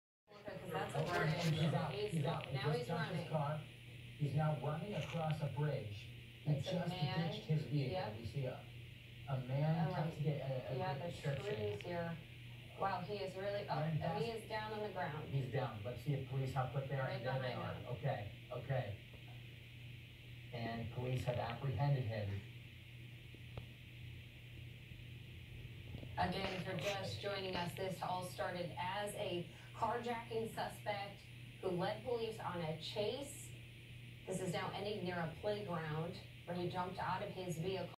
End of high speed chase in Independence, MO
This is the end of a high speed chase that started in Kansas City MO but ended up in Independence MO.